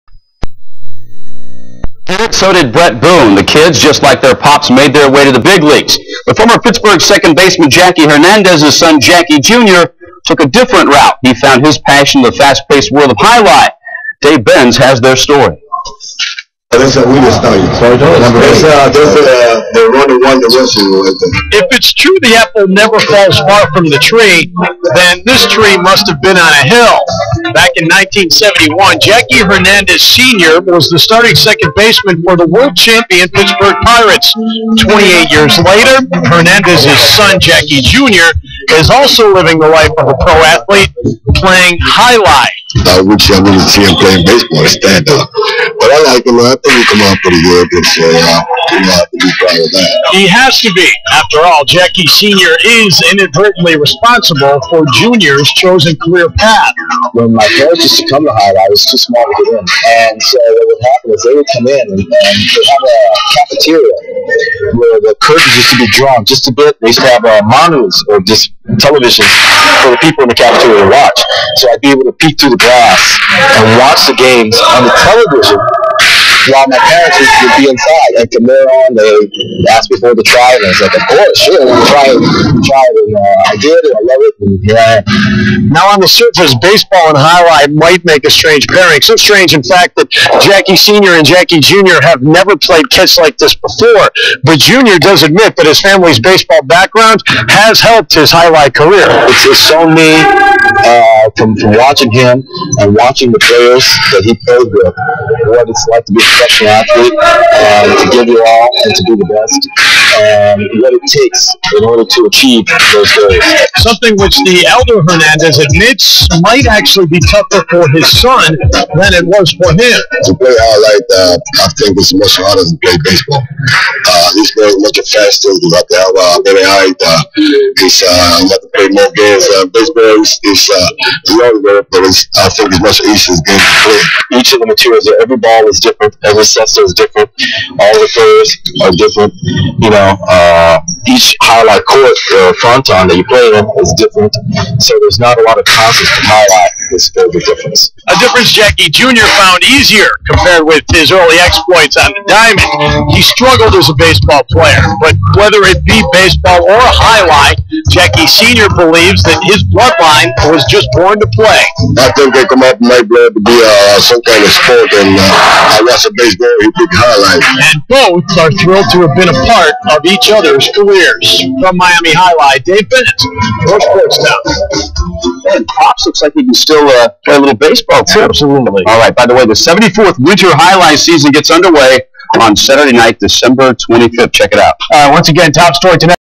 Wami TV Jai - Alai Interview Jackie and father
Wami Jai Alai Interview Jackie Hernandez and his father pittsburg pirates shortstop Jackie Hernandez